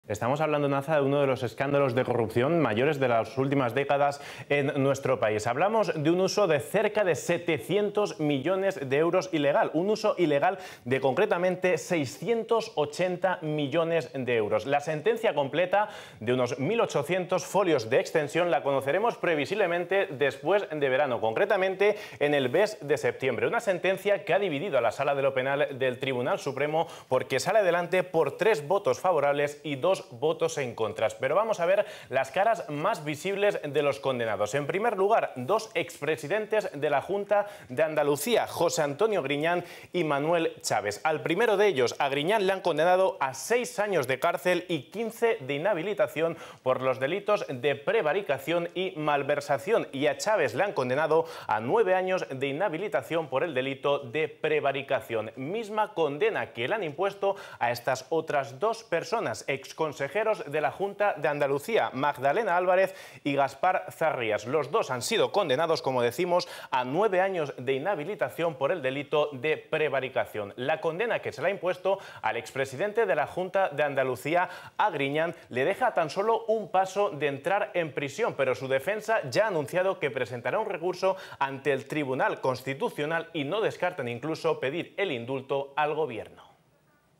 PANTALLÓN | El Supremo confirma la prisión para Griñán y la inhabilitación para Chaves
Pantallón realizado por Víctor Casas para 'TRECE Al Día'.